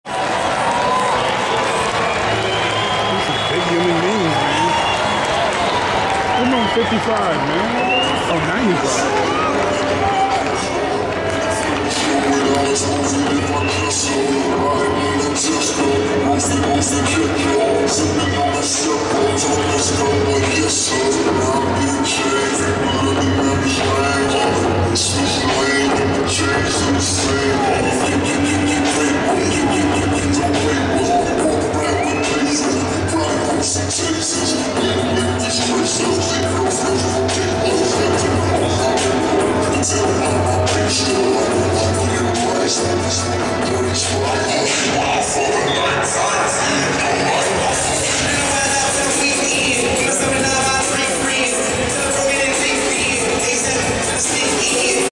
JETS VS GIANTS PRESEASON 2013 clip 2
Giants running out on the field